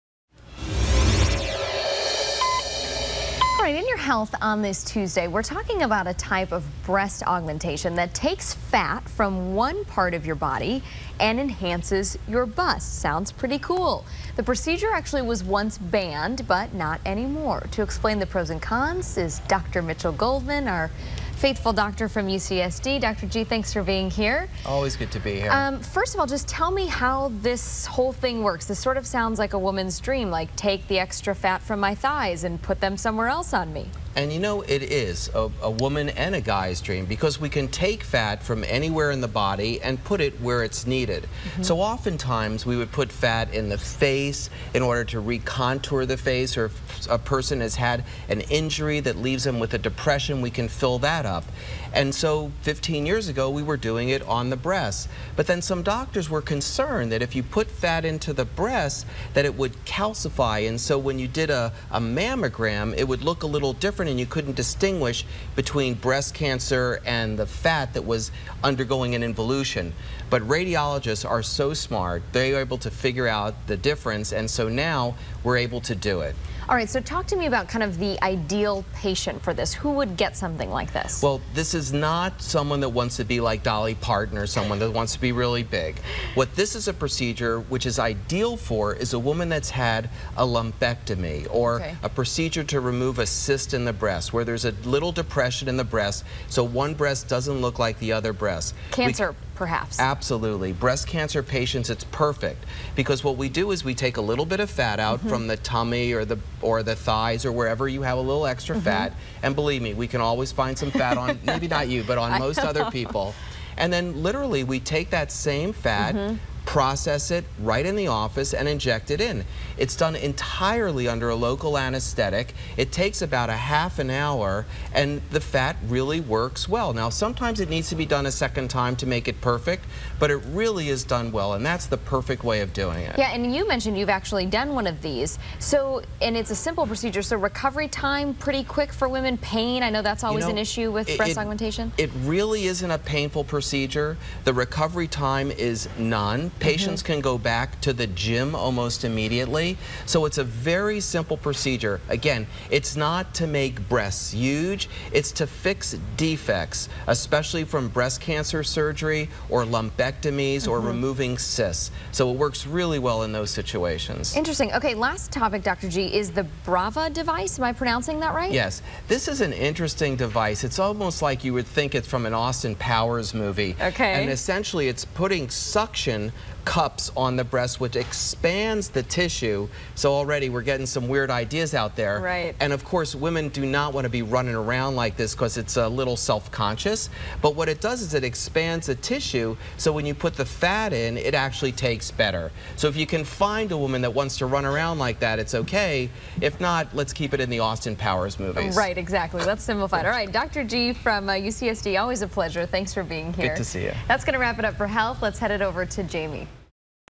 Breast Augmentation Fat Transfer | Mitchel P. Goldman, MD
In this video clip, Dr. Goldman discusses breast augmentation surgery with fat transfer. This procedure involves removing the fat from one area of the body and using it to enhance the breasts. Breast augmentation with fat transfer is ideal for a women who has had a lumpectomy as a result of breast cancer. This procedure is done under local anesthesia and takes only about 30 minutes

To find out if you're a good candidate for breast augmentation fat transfer surgery, contact Dr. Mitchel Goldman today by calling .